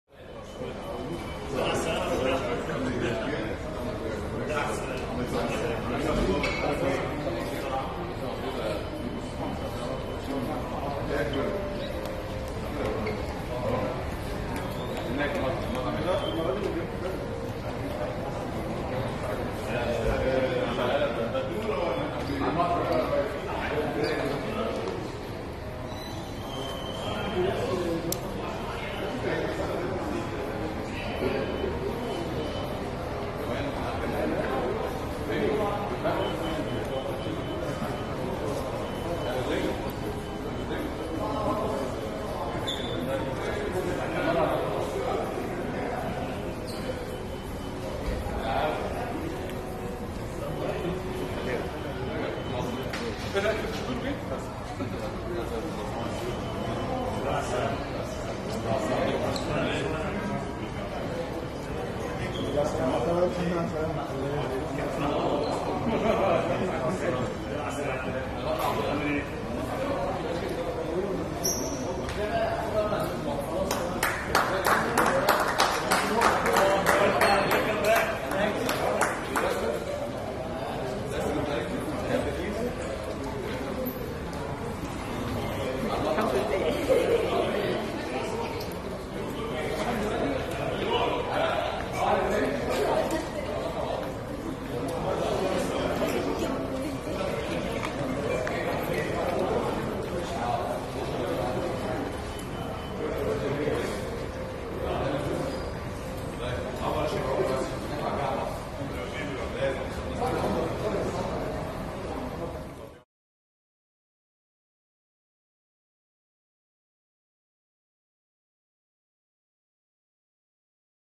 وصول منتخب مصر لكرة اليد للغردقة لخوض معسكر استعداداً للمونديال
تصوير: ريهام حمدي